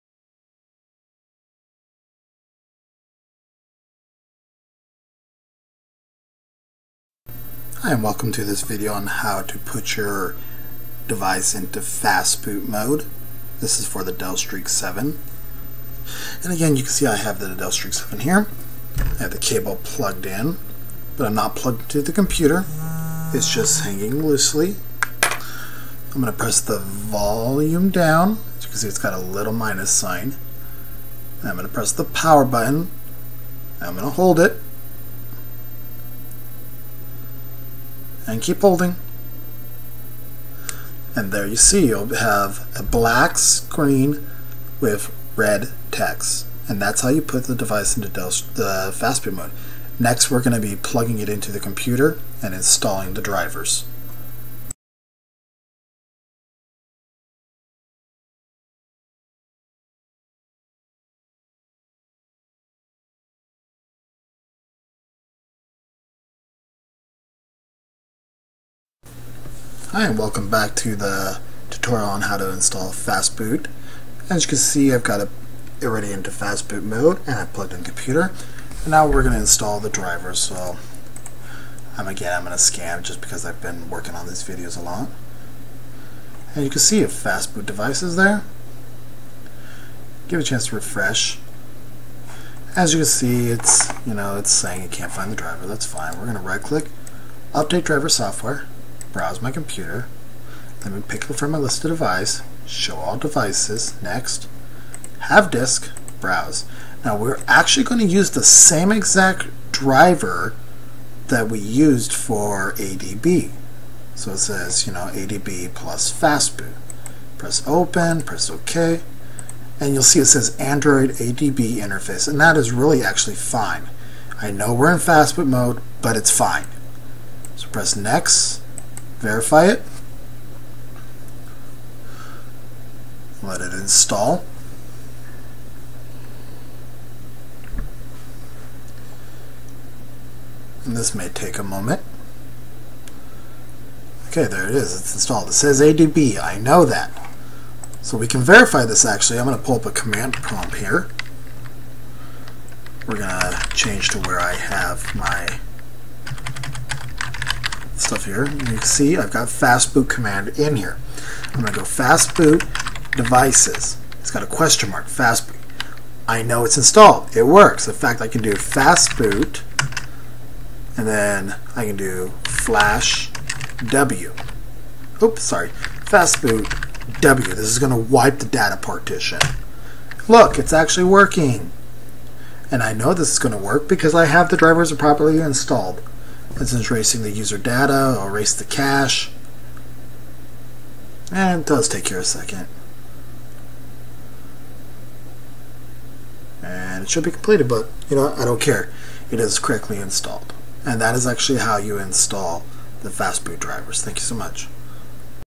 How to put Dell Streak 7 in Fastboot mode
A much improved video on placing the Dell Streak 7 in fastboot mode and installing the drivers on a Windows 7 (x64) desktop